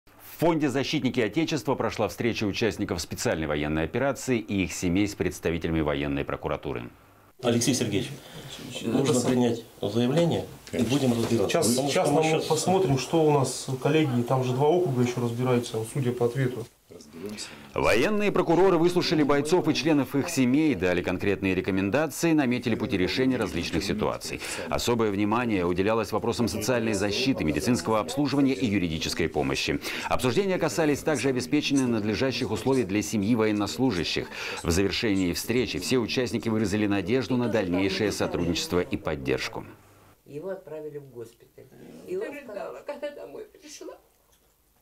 Участники СВО встретились с представителями военной прокуратуры
В фонде «Защитники Отечества» прошла встреча участников специальной  военной операции и их семей с представителями военной прокуратуры.

«Алексей Сергеевич, нужно принять заявление и будем разбираться. Сейчас посмотрим, что у нас еще у коллег, там же два округа еще разбираются, судя по ответу».

Военные прокуроры выслушали бойцов и членов их семей, дали конкретные рекомендации и наметили пути решения различных ситуаций. Особое внимание уделялось вопросам социальной защиты, медицинского обслуживания и юридической помощи. Обсуждения касались также обеспечения надлежащих условий для семьи военнослужащих.  В завершение встречи все участники выразили надежду на дальнейшее сотрудничество и поддержку.